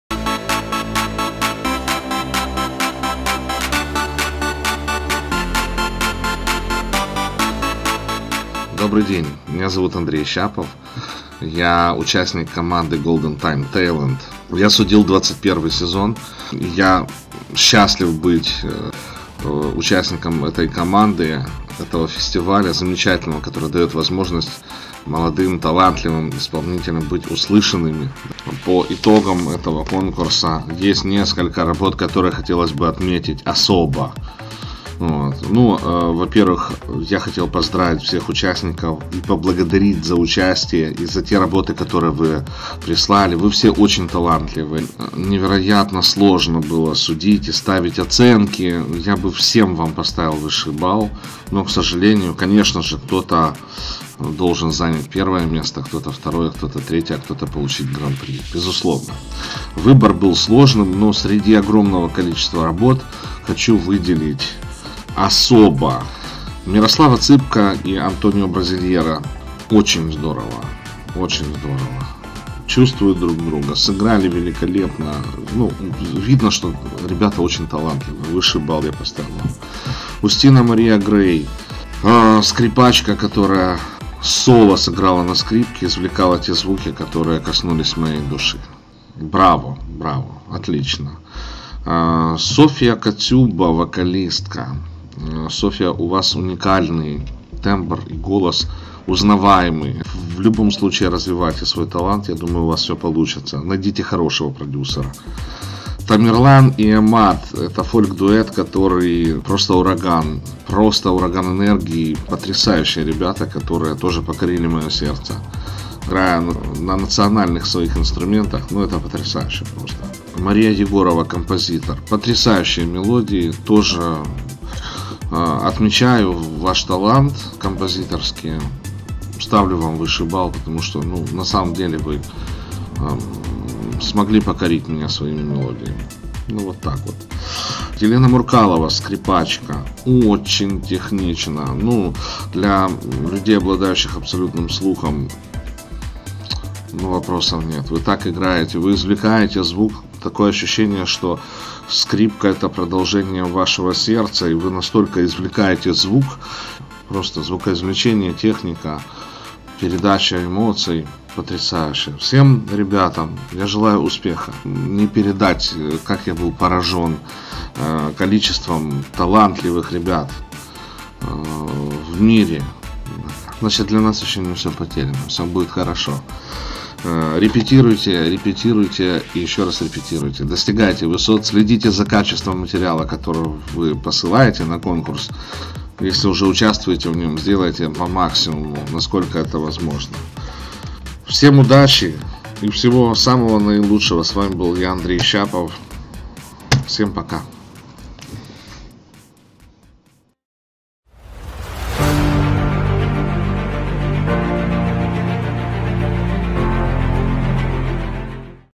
Видео оценка конкурсных работ 21 сезона - жюри конкурса Golden Time Talent - Andrey Shchapov 🇺🇦
Дорогие друзья!
Публикуем видео оценку жюри конкурса Golden Time Talent - Andrey Shchapov 🇺🇦  Ukraine